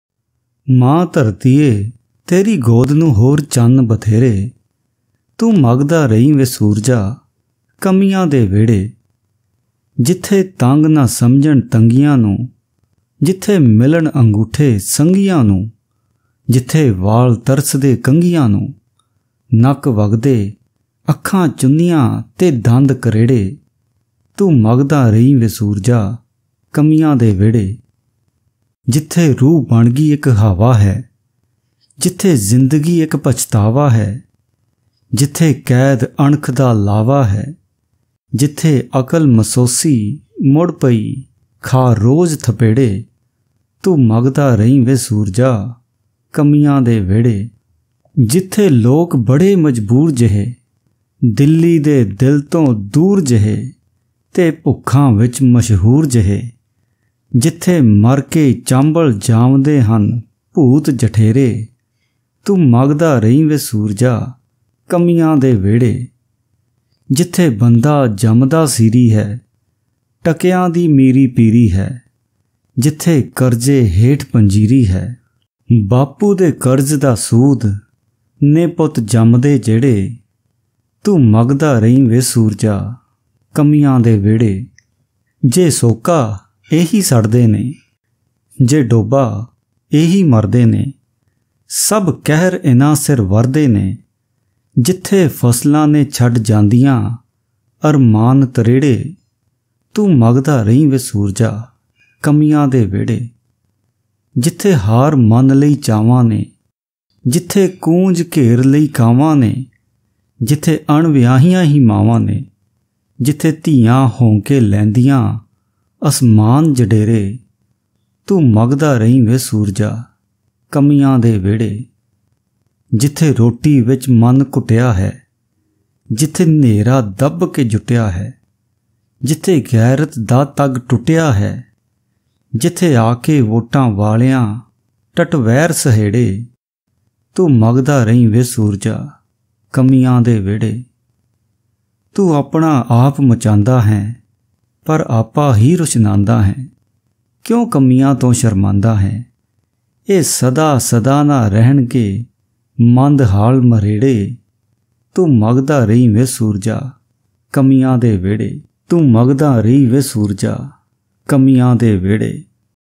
ਕੰਮੀਆਂ ਦਾ ਵਿਹੜਾ | ਸੰਤ ਰਾਮ ਉਦਾਸੀ
Recitation by Tajinder Singh